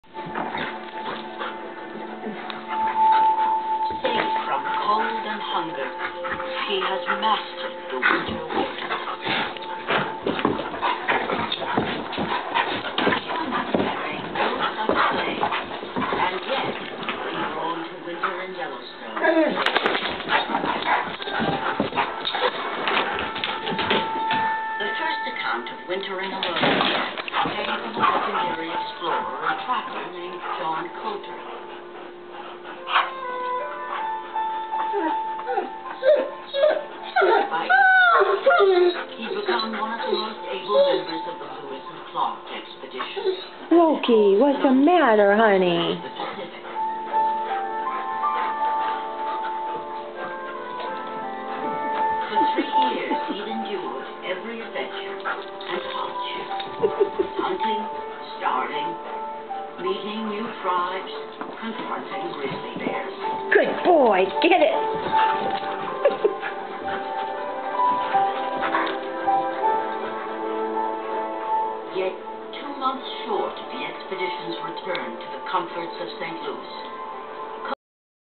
Best friends celebrating Christmas together.avi
Vixen the little German Shepherd Dog  (18 mos old) & Loki the Weimaraner (8 yrs old) are truly bestest friends. Loki's parents couldnt take him with them this Christmas when they left town & thought he might enjoy spending it w/ his bestest doggy friend i the whole world which was the best gift they could've given him & Vixen.